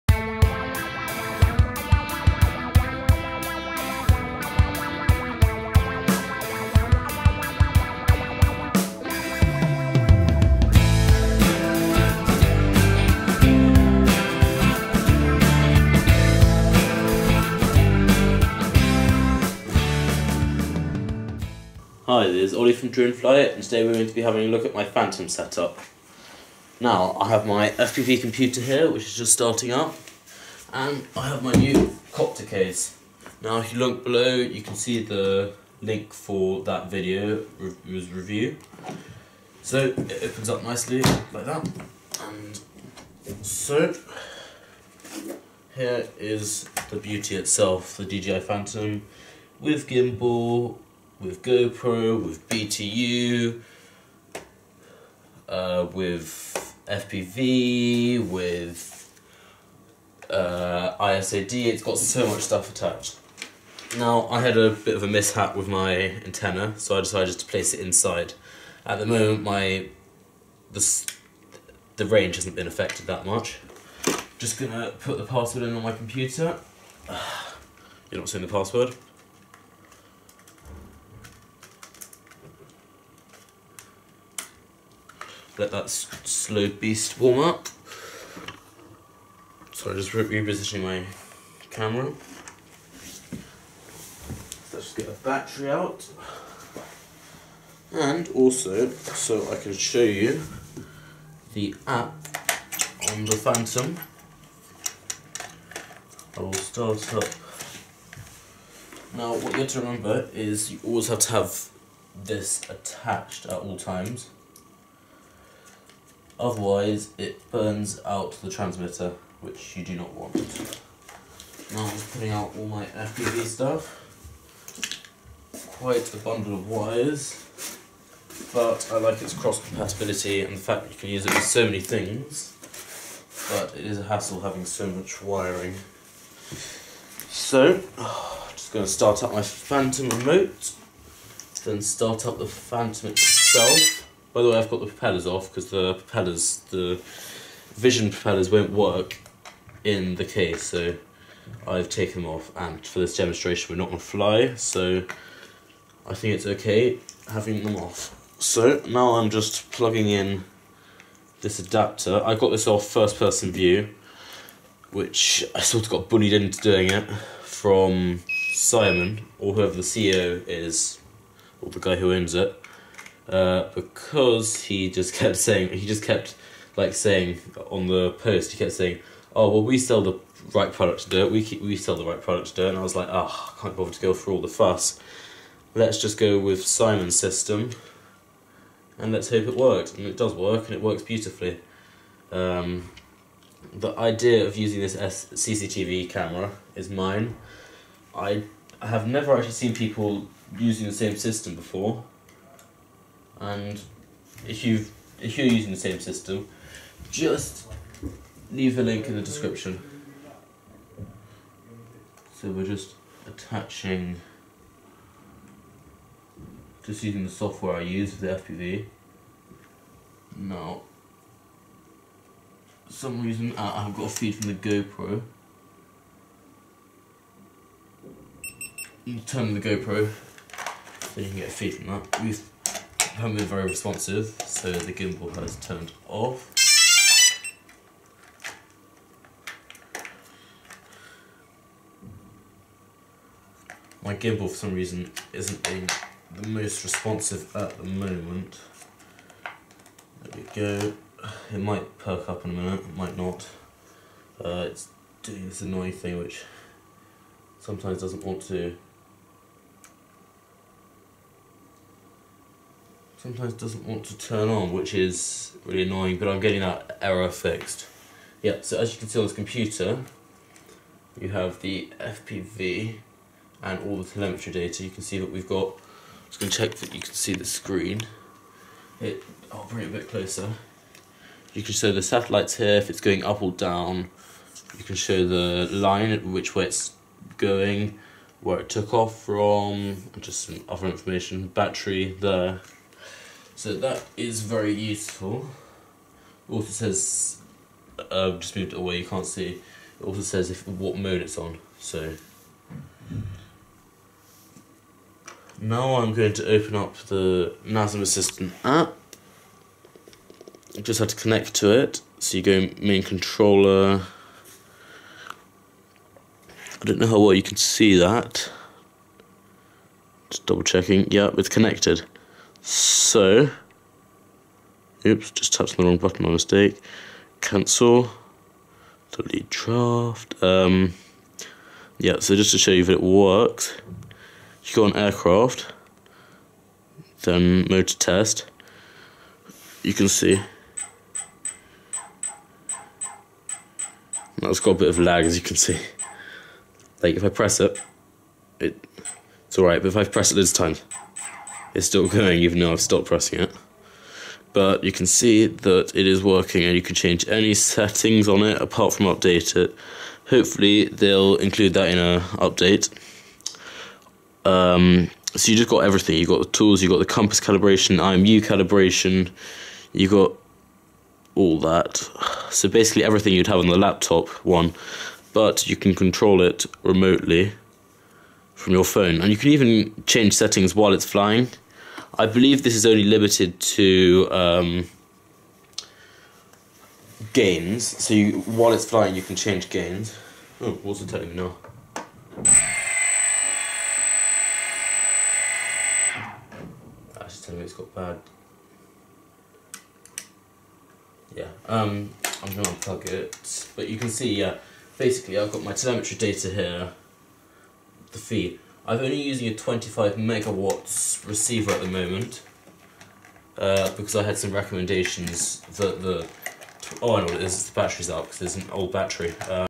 Fully Upgraded Dji Phantom 1.1.1
This is my Dji Phantom 1.1.1 with upgrade board, isod mini, zemuse H3-2D gimbal, Immersion RC FPV Transmitter, and a  GoPro Hero 3+ Black Edition. In this video I will show you how it all works and how some of the wiring works.

To Donate And Help Me Afford New Soft Ware And Equipment